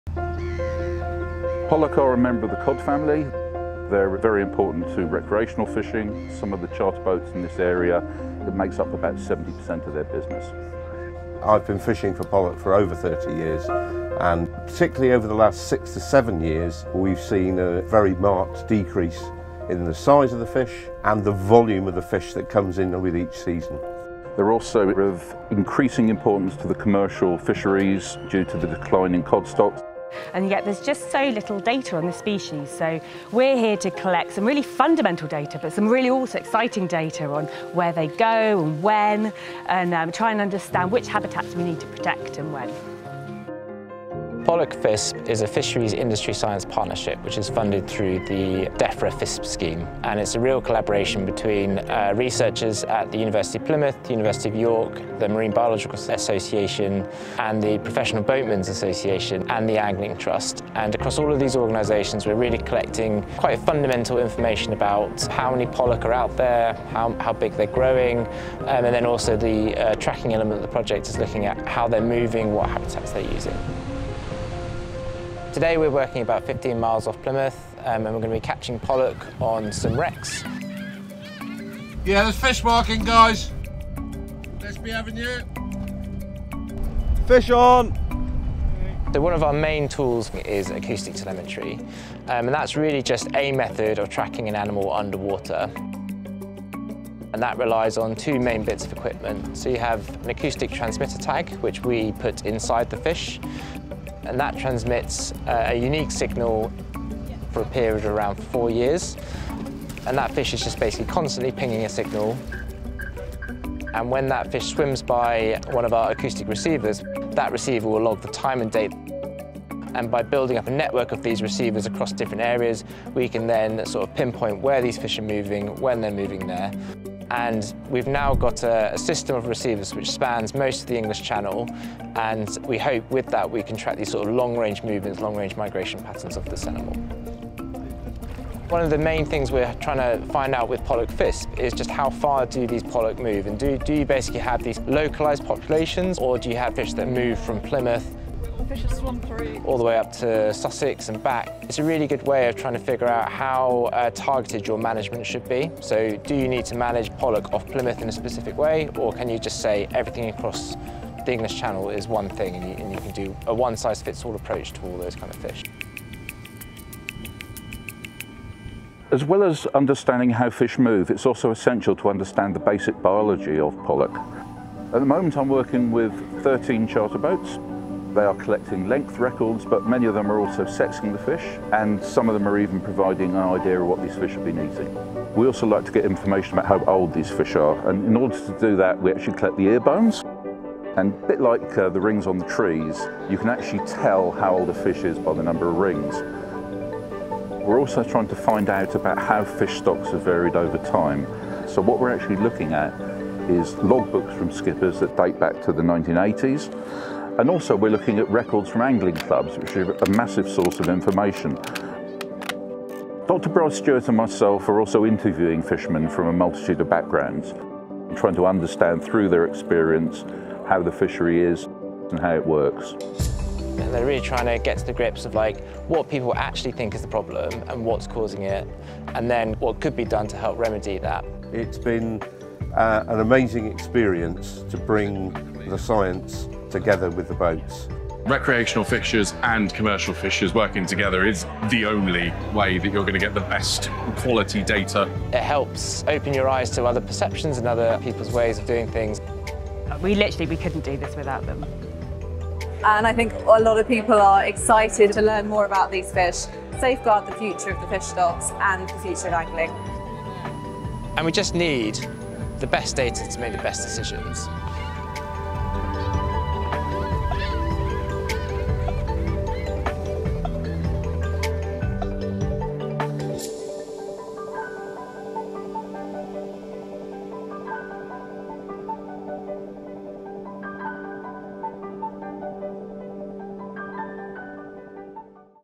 Pollack FISP
A Marine Conservation Research Group project to collect vital data about pollack movement, habitat choice, populations and life history.